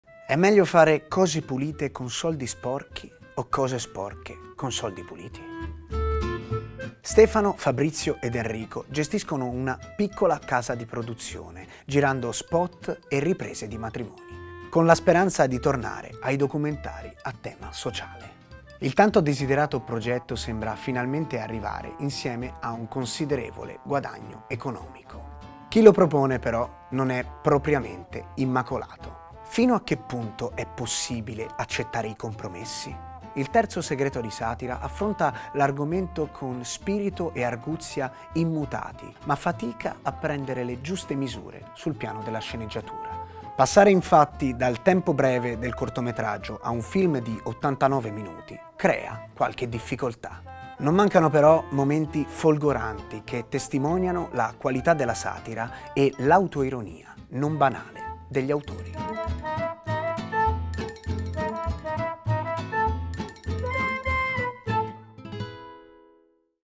Si muore tutti democristiani (2018) - MYmovies.it
Commedia con alcuni momenti folgoranti che testimoniano della qualità della satira e dell'autoironia non banale degli autori. Recensione di Giancarlo Zappoli, legge Lorenzo Frediani.

Stefano, Fabrizio ed Enrico gestiscono una piccola casa di produzione girando spot e riprese di matrimoni, con la speranza di tornare ai documentari a tema sociale. Il tanto desiderato progetto sembra finalmente arrivare insieme a un considerevole guadagno economico. Chi lo propone però non è propriamente immacolato.

Fino a che punto è possibile accettare i compromessi? Il terzo segreto di satira, al primo lungometraggio, affronta l'argomento con spirito e arguzia immutati, ma fatica a prendere le giuste misure sul piano della sceneggiatura. Passare infatti dal tempo breve del cortometraggio a un film di 89 minuti crea qualche difficoltà. Non mancano però momenti folgoranti che testimoniano della qualità della satira e dell'autoironia non banale degli autori.

Regia di Il Terzo segreto di Satira. Un film con Marco Ripoldi, Massimiliano Loizzi, Walter Leonardi, Renato Avallone, Valentina Lodovini.
Genere Commedia - Italia, 2017, durata 89 minuti. Uscita cinema giovedì 10 maggio 2018 distribuito da 01 Distribution.